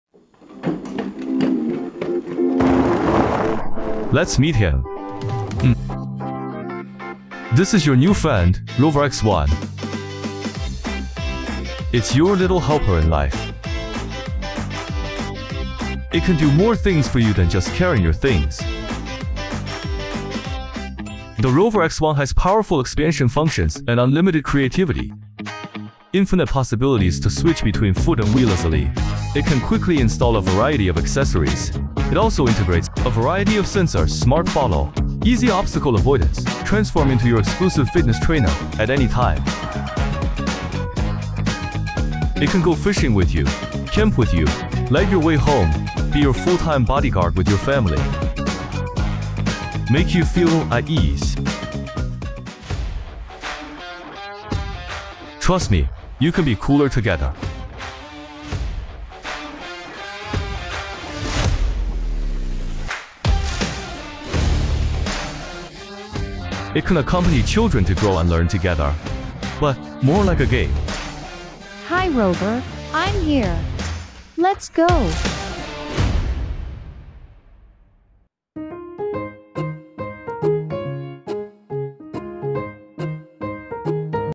Rover X1 Robot Dog from DOBOT (越疆) debuts at $1,050 USD
It uses dual‑vision tracking for omnidirectional perception, an all‑terrain wheel‑leg chassis with load‑carrying ability, and an open platform for roles like follow‑cam, home security, programming education and companionship. Would you buy a robot dog that follows you and hauls your gear? 👀👇 (unofficial English translation via CapCut)